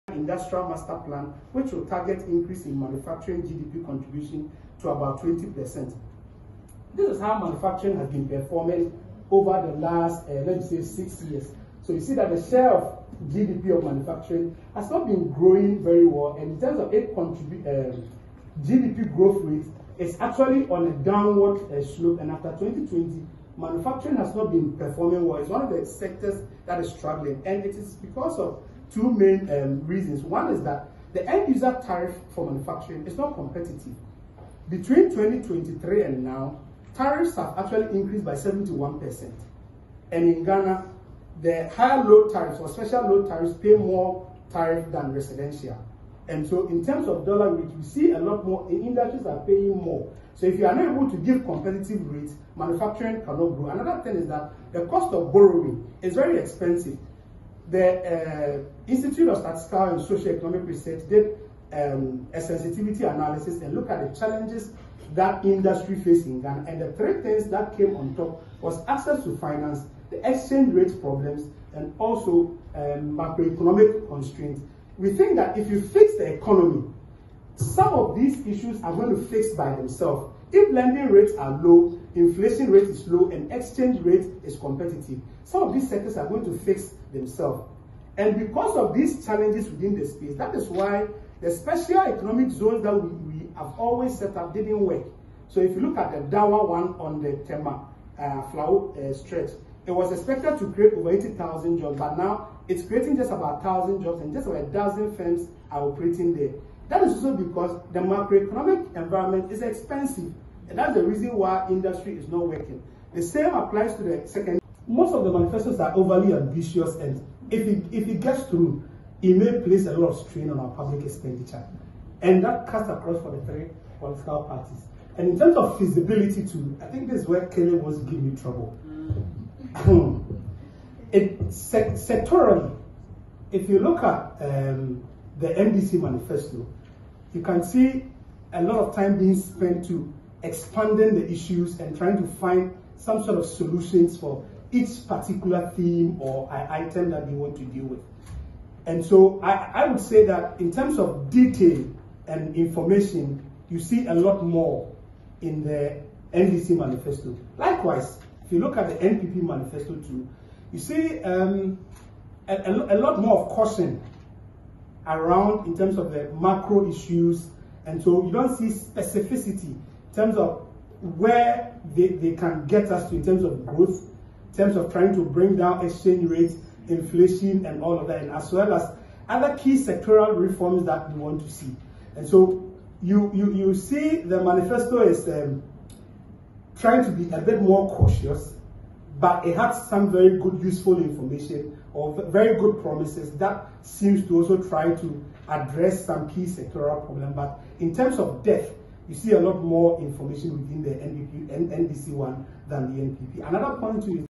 IMANI Expresses Concerns About Party Manifestos
The IMANI Centre for Policy and Education has expressed concerns that the industrial master plan policy outlined in the manifestos of major political parties is impractical due to Ghana's unfavorable economic climate...